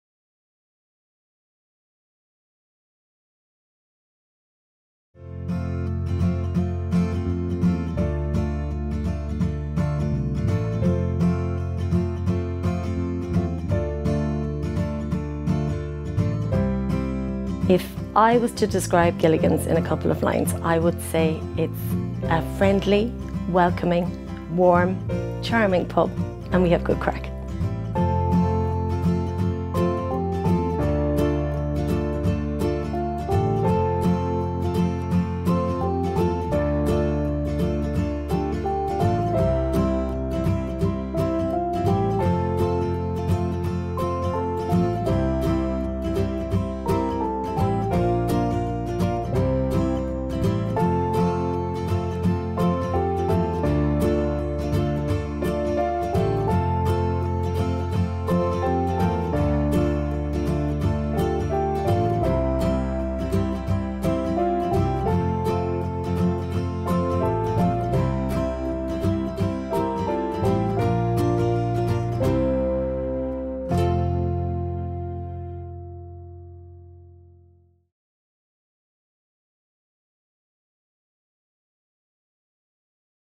Gilligans Bar, Claremorris
Another pub we visited was Gilligans Bar in Claremorris, Co.Mayo. A small pub with a big personality and plenty of entertainment!